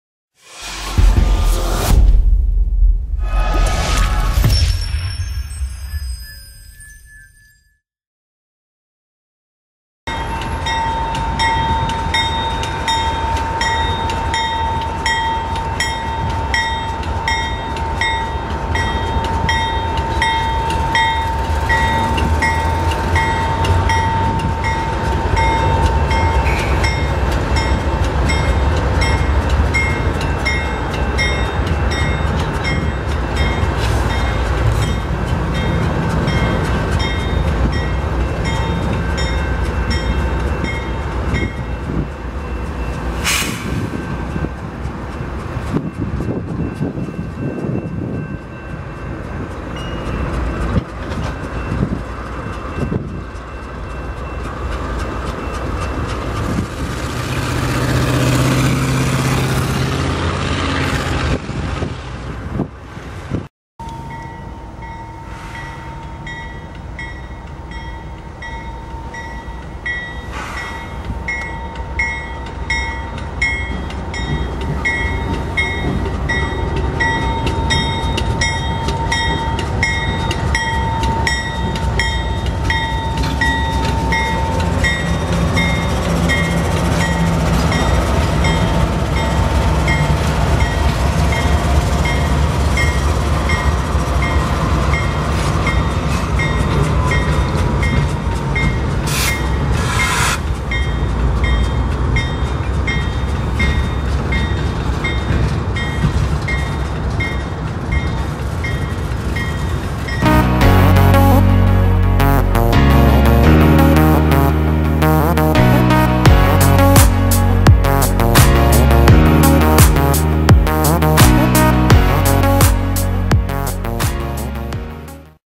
CP Head End Locos at Grade Crossings in North Minneapolis
The Drews Trains was out N about on Presidents Day 2019.  This is a short clip of the same train at 2 different crossings. We do not have much footage from this area or of CP so this was neat to have a mini-chase!